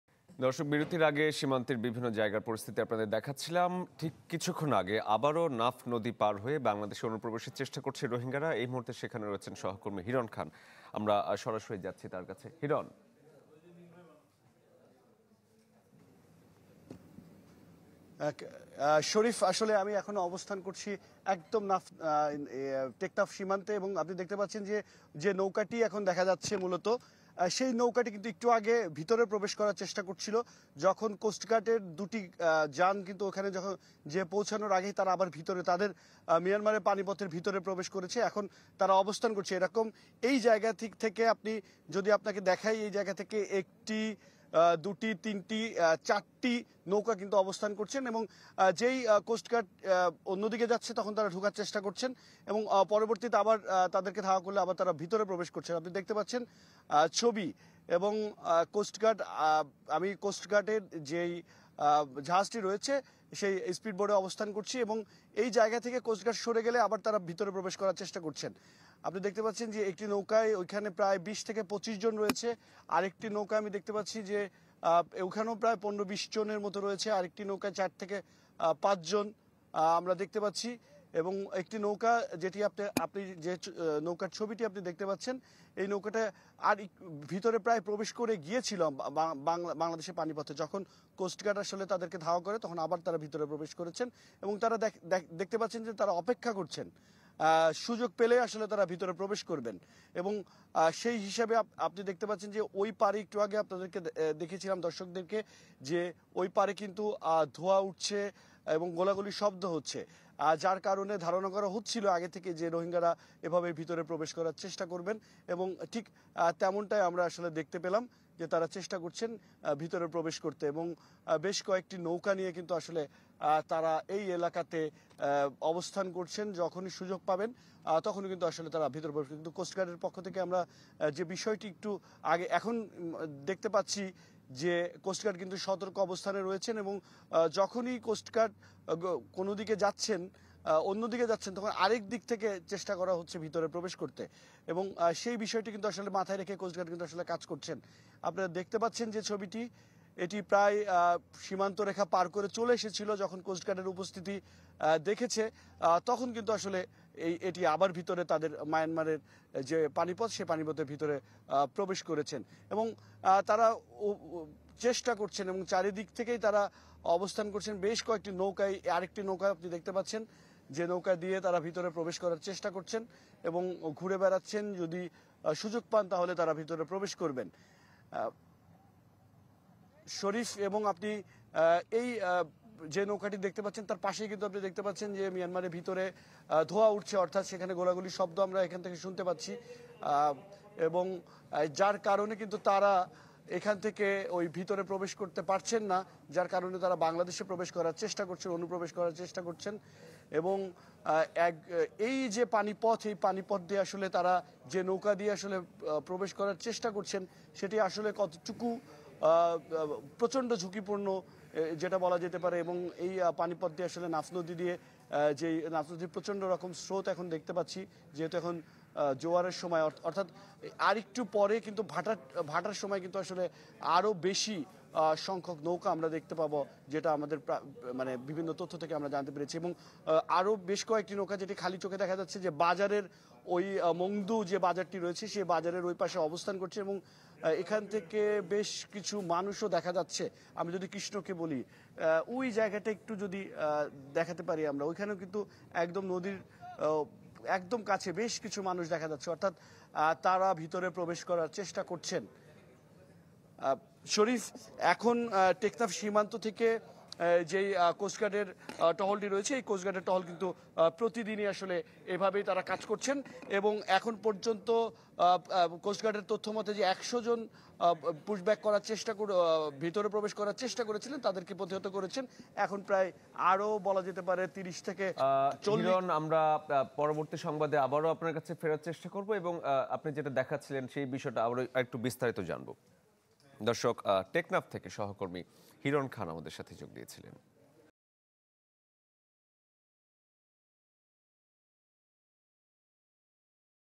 Exclusive: চ্যানেল 24 এর ক্যামেরায় রোহিঙ্গা অনুপ্রবেশের ভিডিও | Myanmar Bangladesh Border | Rohingya
»» Office Address:
Bangladesh.